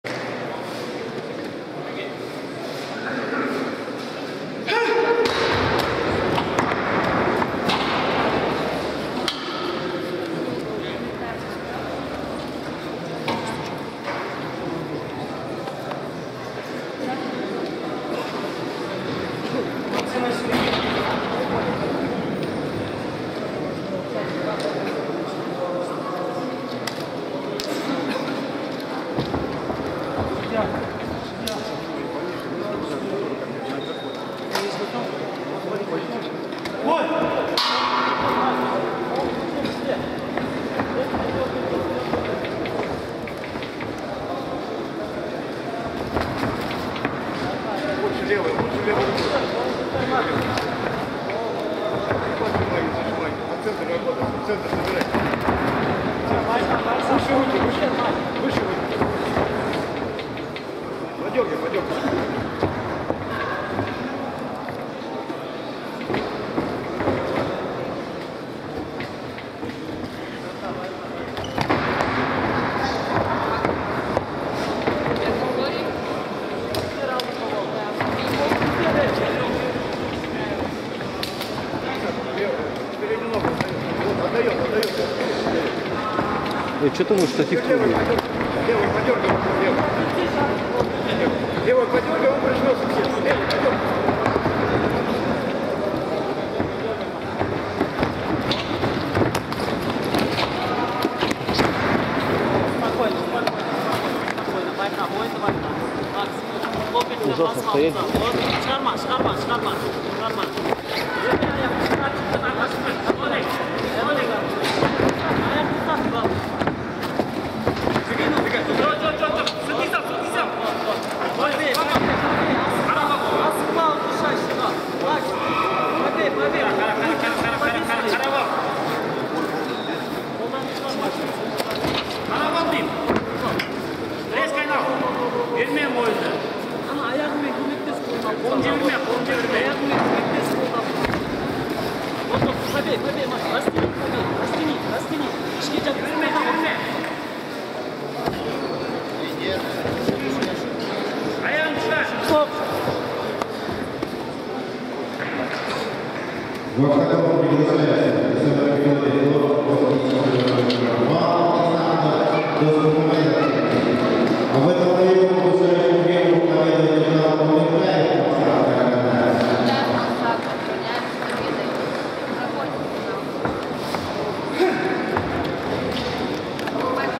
IV Кубок Казахстана по ММА. 77,1 кг Омиртаев Максат - Жумабай Толеген
IV Кубок Казахстана по ММА. 77,1 кг Омиртаев Максат (Карагандинская область) - Жумабай Толеген (Карагандинская область)